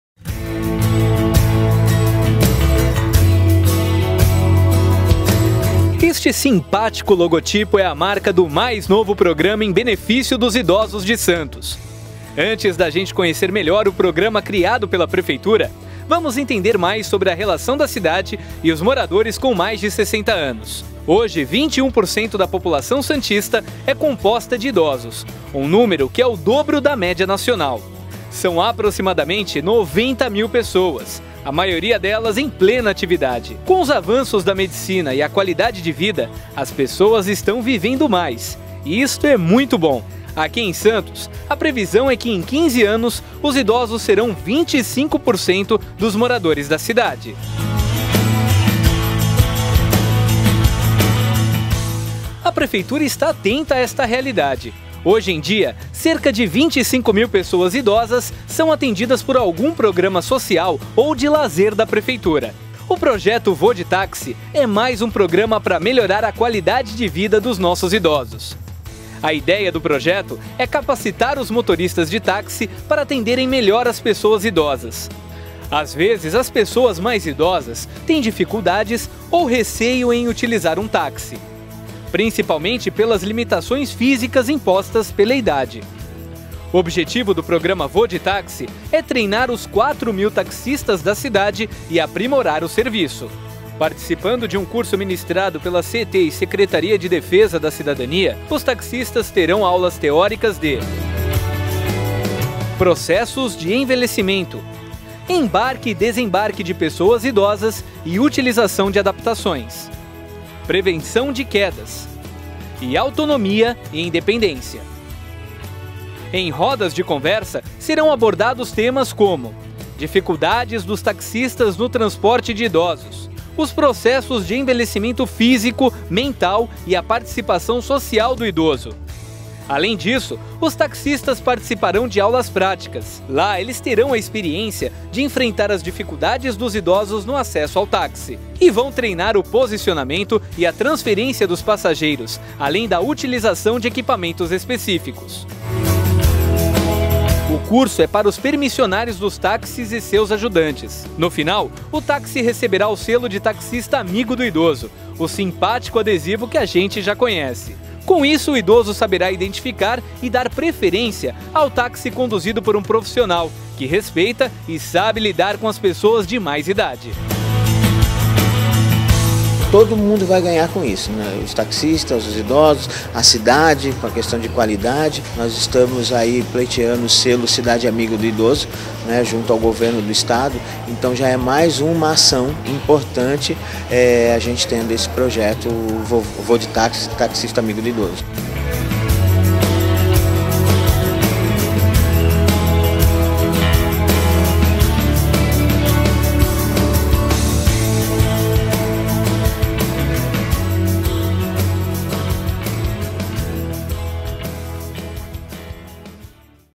Programa ‘Vô de Táxi’ capacita taxistas no transporte de idosos
Na Cidade vivem cerca de 85 mil idosos (cerca de 20% da população total), muitos deles com dificuldades de locomoção. Pensando no conforto deles é que a Prefeitura lançou nesta terça (19), o novo projeto.